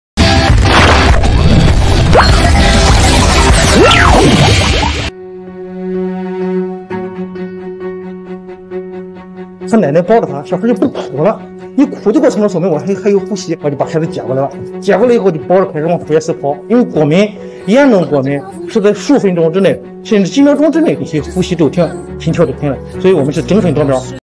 医生一路狂奔8分钟救回窒息婴儿 上演“生死时速”
近日，山东潍坊，一名5个月大的婴儿吃蛋黄引发严重过敏休克窒息。医生从家长手中接过孩子后，一路狂奔到输液室进行抢救。经过8分钟左右的“生死急救”，孩子情况转危为安。网友：谢谢你，奔跑的天使！